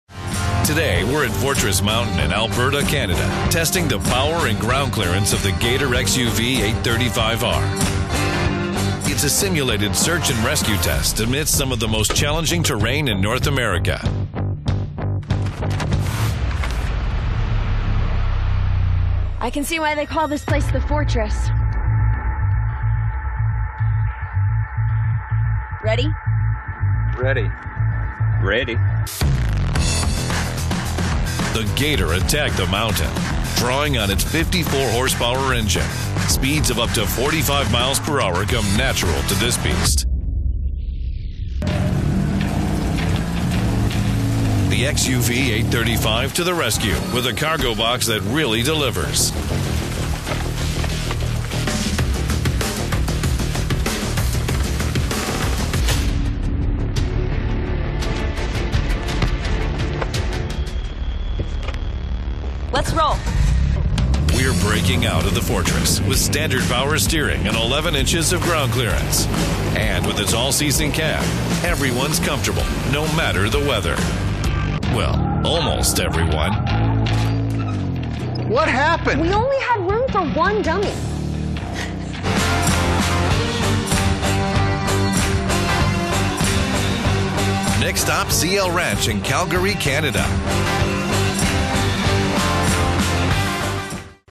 Fortress Mountain | John Deere Gator™ XUV835
The Great North American Test Drive heads to the massive Fortress Mountain in the province of Alberta. See how the team, with the help of the powerful Gator(TM) XUV835, performs a simulated search and rescue of a backcountry hiker.  Now it's your turn to take a test drive.